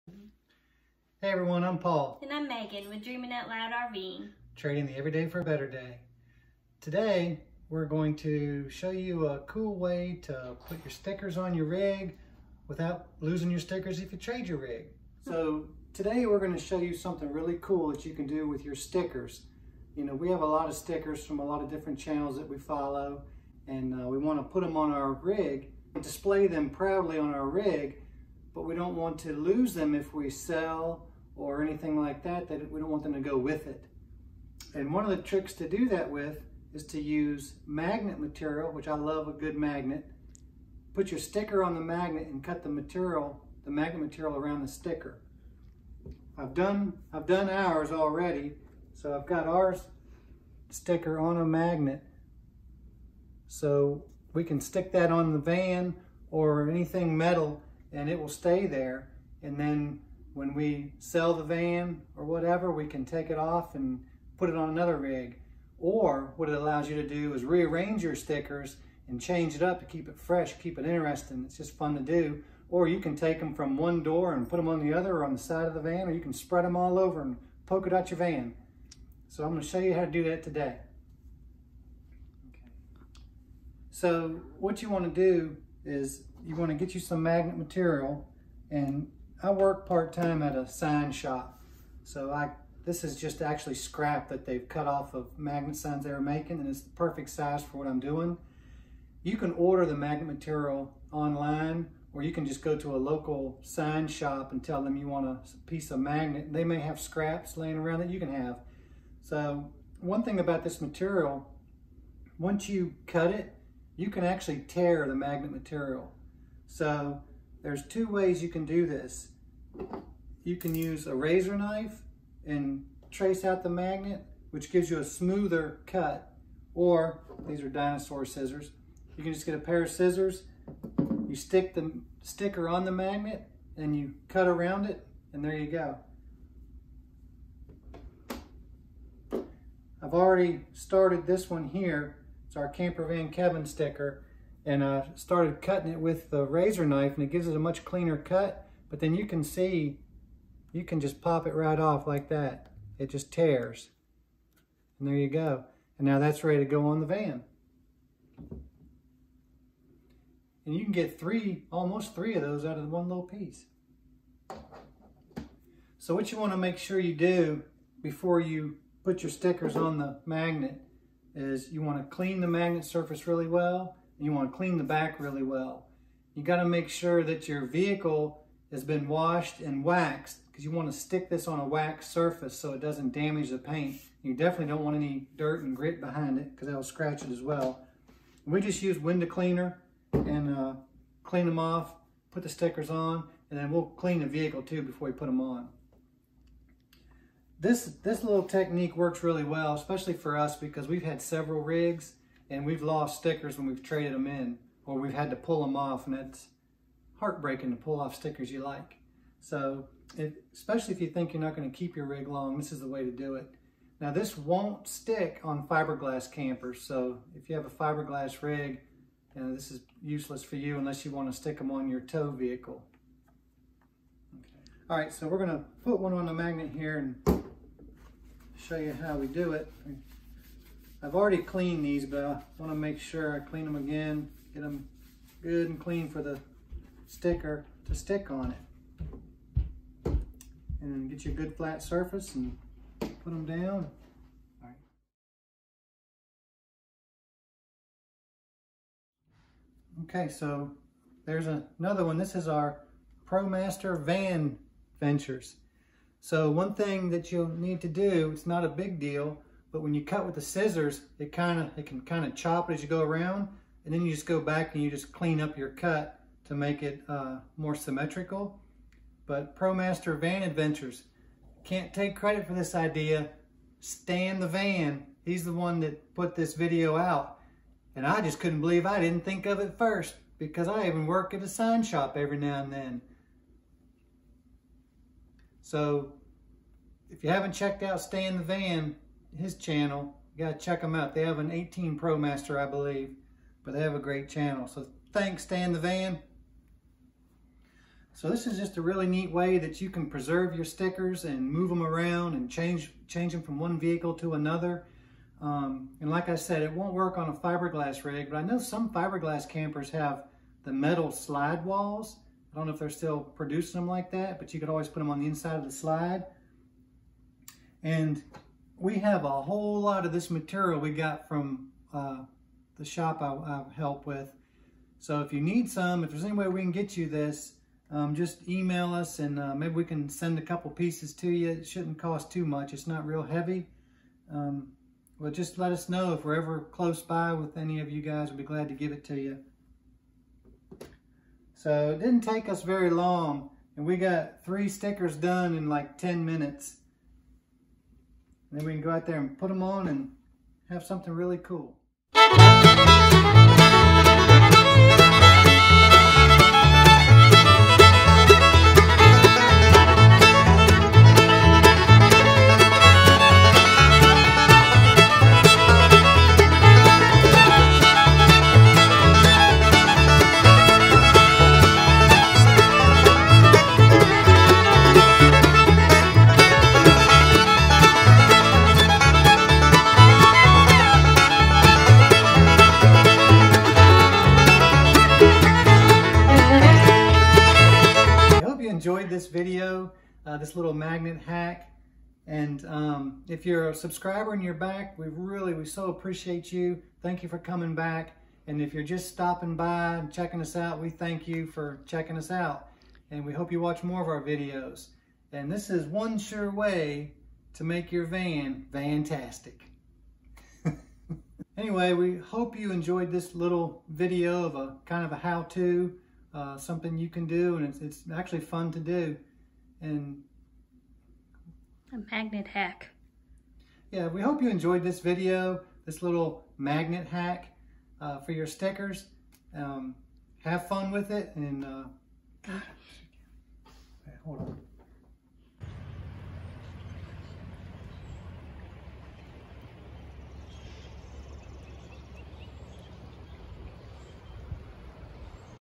Channel Stickers Magnetized! Van Life
This is a how to video on preserving your stickers on your rig. Weather you display them on the exterior or inside on your refrigerator. We show you how to apply them to magnetic material so they can be moved around and taken off a camper or van that you may be getting rid of, now you won't loose your favorite stickers.